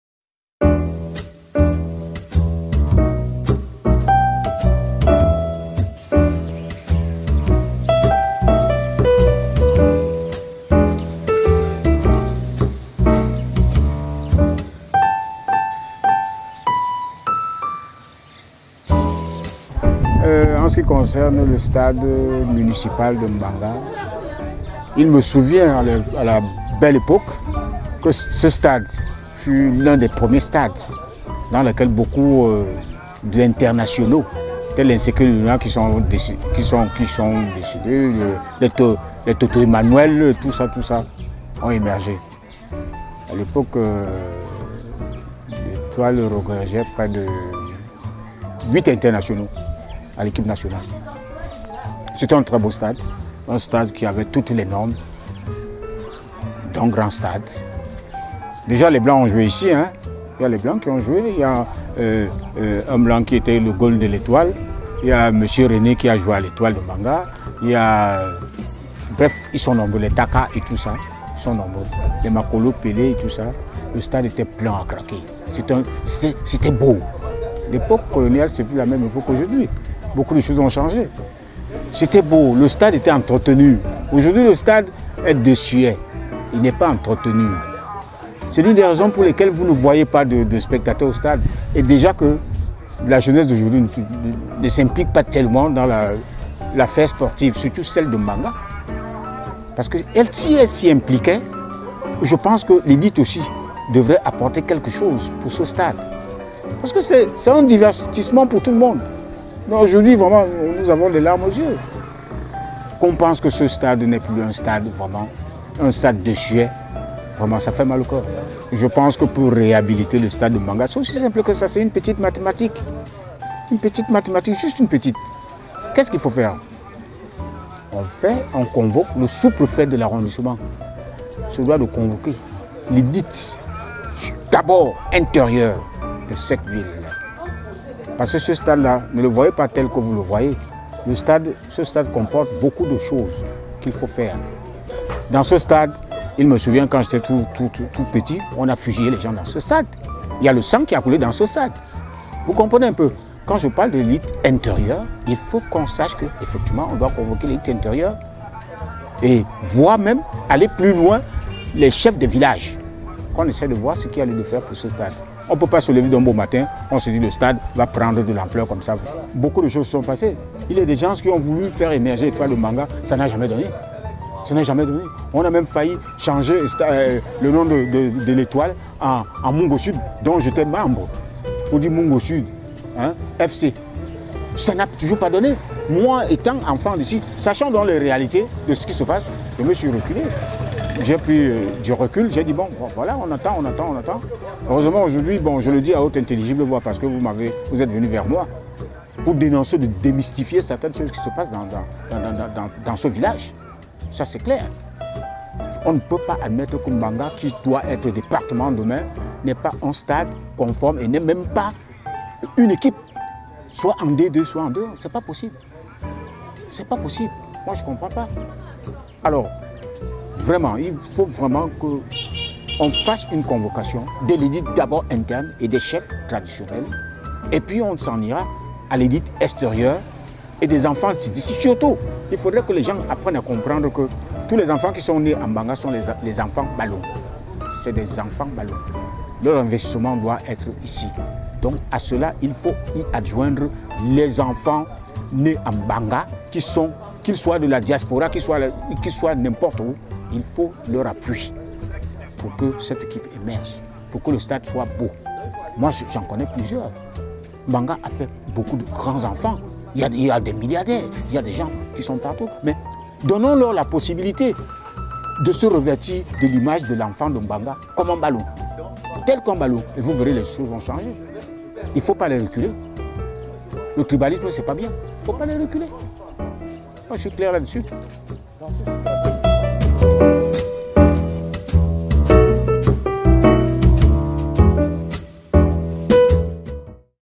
L'HISTOIRE DU STADE MUNICIPAL DE MBANGA - CANAL MBANGA PREMIÈRE
Le stade municipal de Mbanga, situé au quartier 15 de Mbanga et derrière la mairie, reste le symbole d'une épopée et surtout le lieu tragique lors de la colonisation. Suivez plutôt cette vidéo.